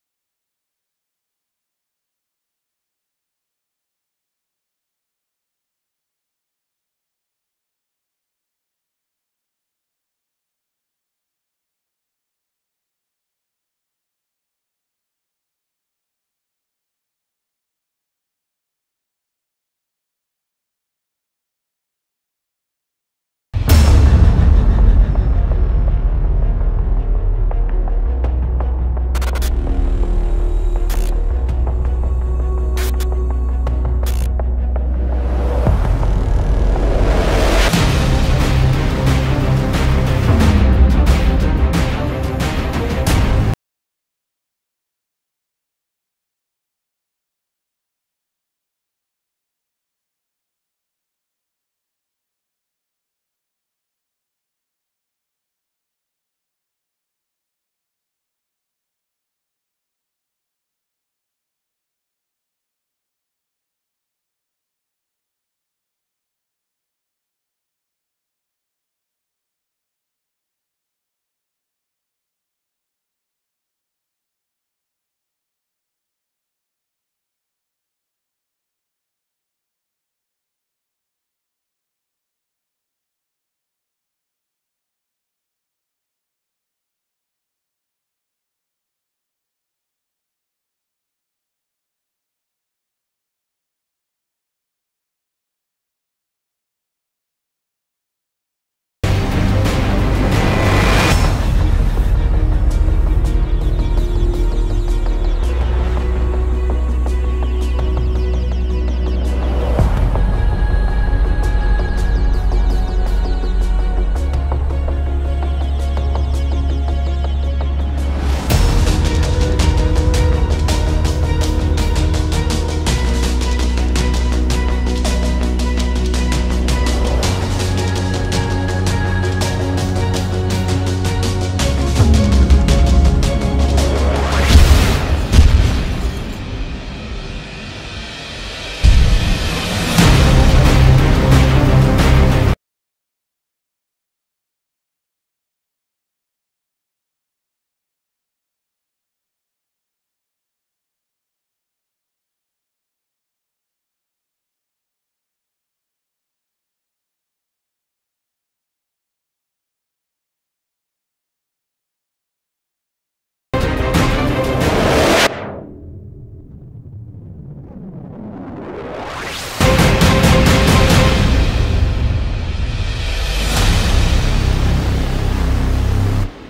RAFAEL’s SEA BREAKER 5th Generation, Maritime & Land Based, Long Range Autonomous Weapon System
SEA BREAKER was developed in order to fill an operational gap in maritime dominance and deep land strike systems ‒ all in a single platform